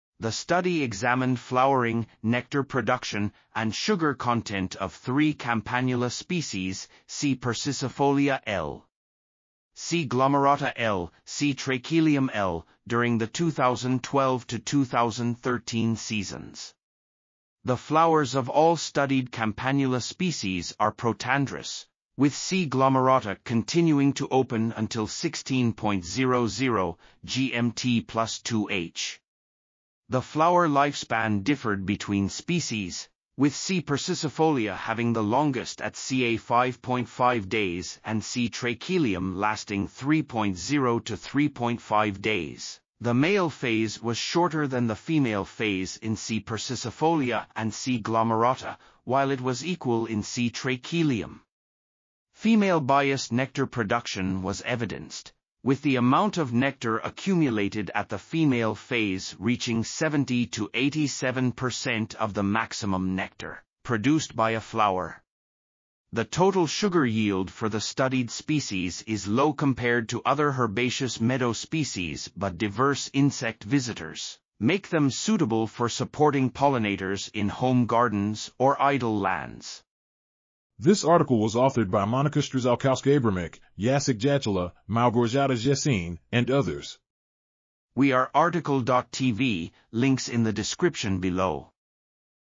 NECTAR PRODUCTION IN SEVERAL Campanula SPECIES (CAMPANULACEAE) | RTCL.TV
### Keywords ###

### Article Attribution ###
Title: NECTAR  PRODUCTION  IN  SEVERAL  Campanula  SPECIES (CAMPANULACEAE)
Authors: Monika Strzałkowska-Abramek, Jacek Jachuła, Małgorzata Wrzesień, Małgorzata Bożek, Agnieszka Dąbrowska ,and Bożena Denisow
Publisher: University of Life Sciences in Lublin - Publishing House
DOI: 10.24326/asphc.2018.3.13

### Video Timestamps ###
0:00:00 - Summary
0:01:20 - Title
0:01:27 - Tail
0:01:32 - End